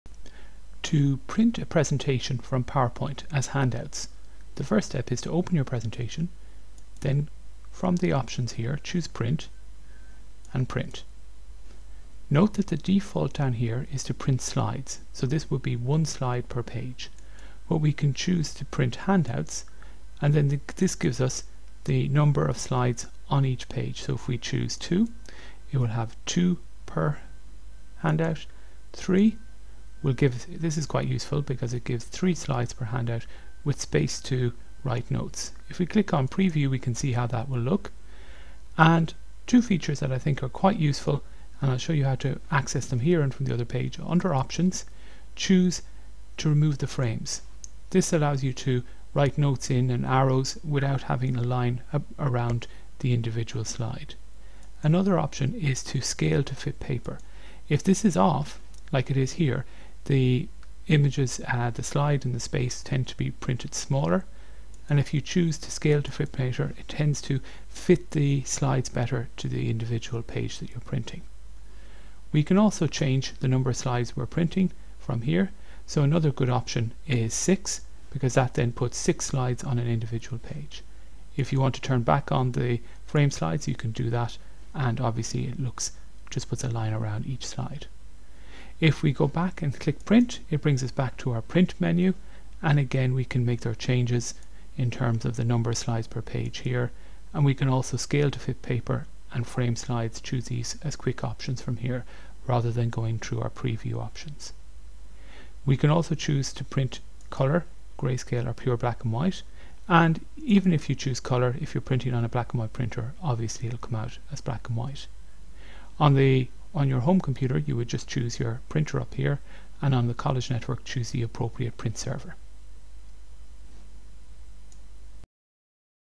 PowerPoint Printing handouts
How to print PowerPoint slides in handout form